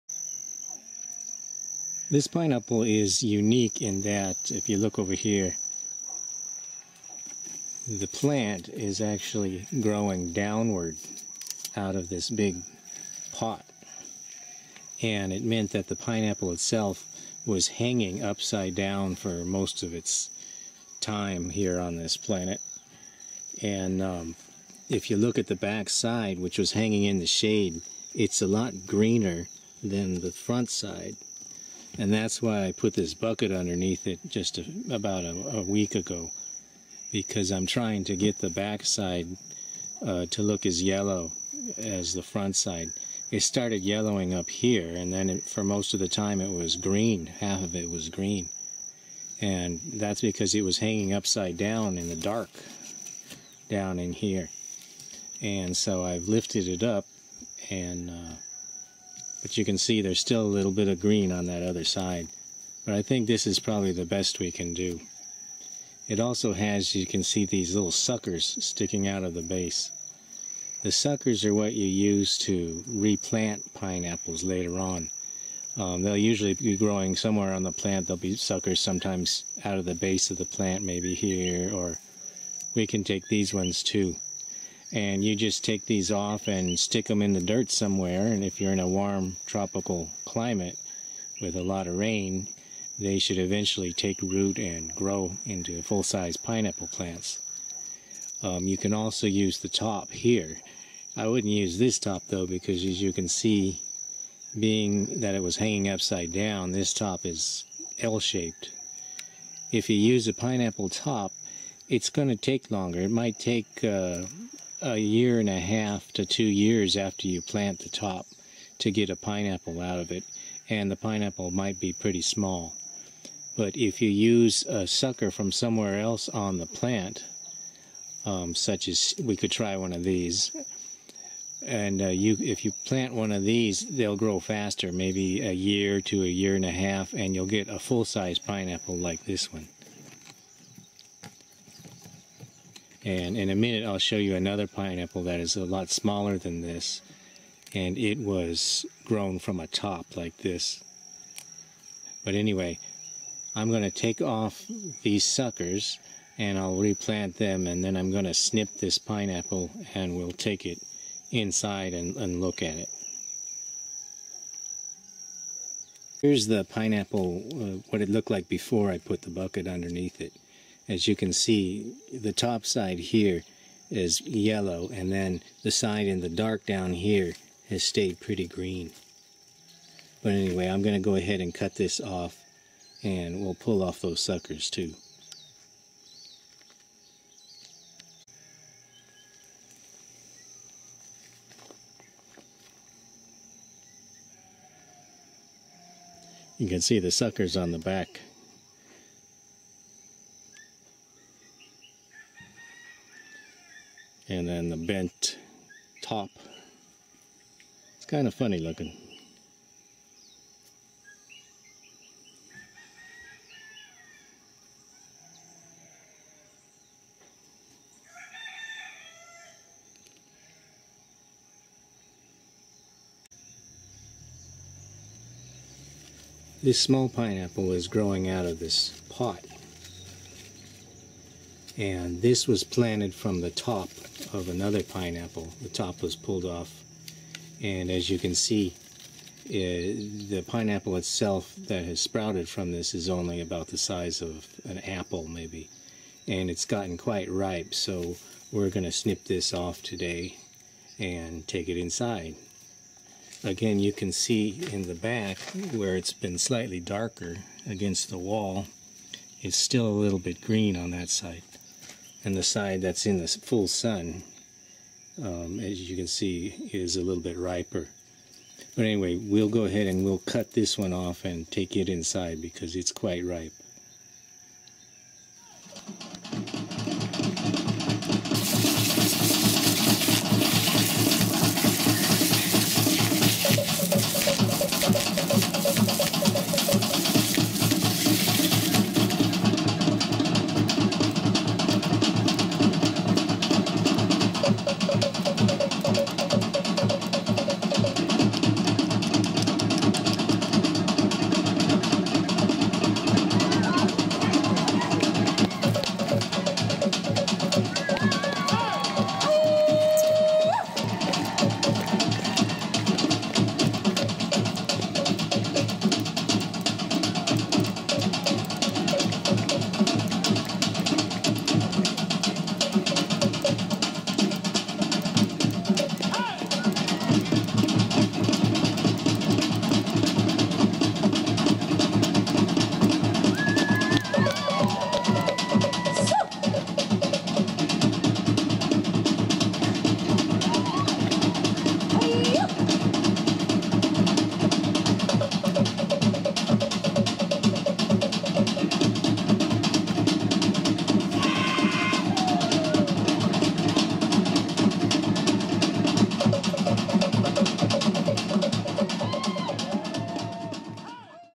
It's Pineapple Season! | Video
- pineapples (Ananas comosus) have an edible multiple fruit consisting of coalesced berries, also called pineapples. In Hawaii, pineapples usually ripen during the wet season.

Pineapples may be cultivated by replanting the crown of the fruit, possibly flowering in 5–10 months and fruiting in the following six months. Replanting suckers will usually result in larger, faster growing fruit. Pineapples do not ripen significantly after harvest.